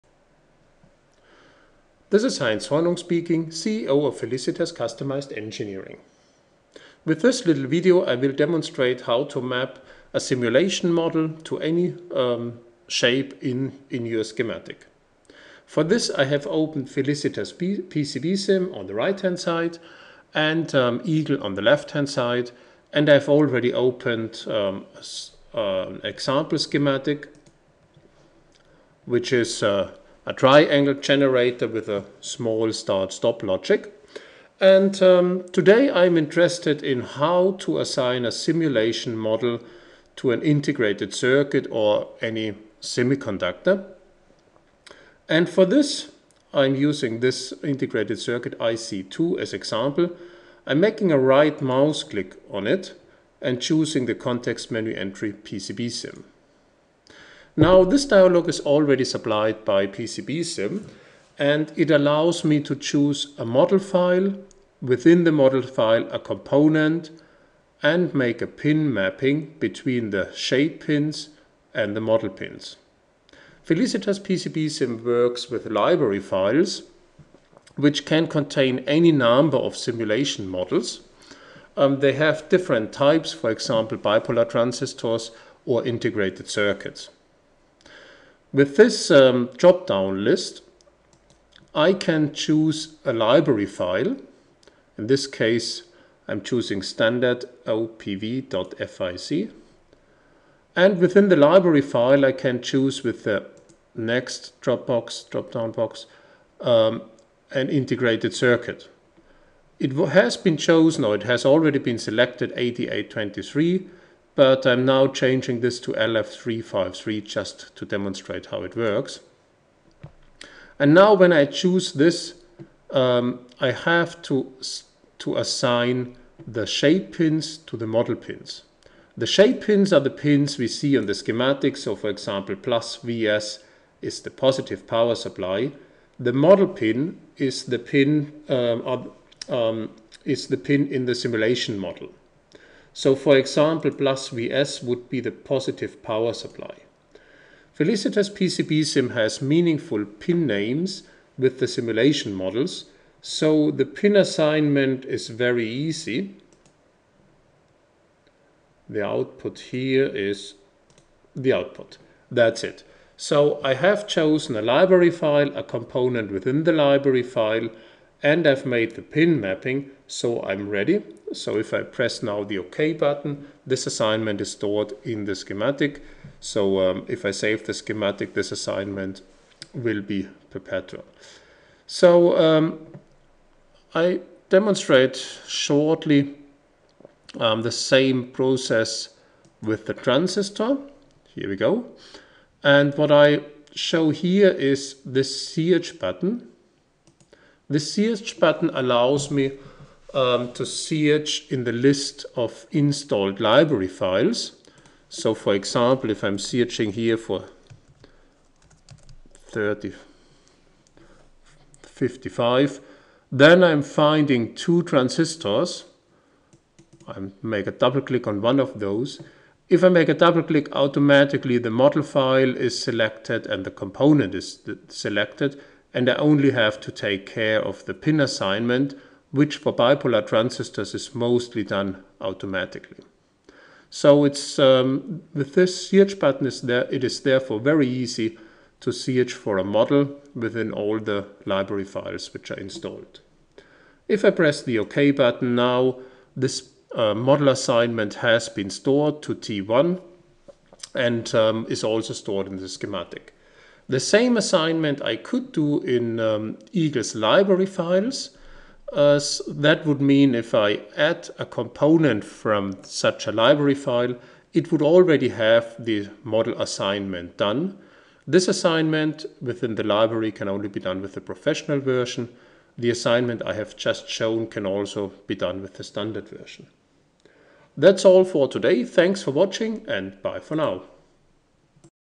Assigning Spice models EAGLE Schematic english
Assigning Spice models to components in the EAGLE Schematic Editor